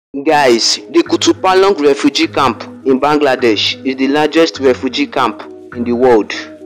The Largest Refugee Camp In The World
Kutupalong refugee camp is the world's largest refugee camp. It is in Ukhia, Cox's Bazar, Bangladesh, inhabited mostly by Rohingya refugees that fled from ethnic and religious persecution in neighboring Myanmar.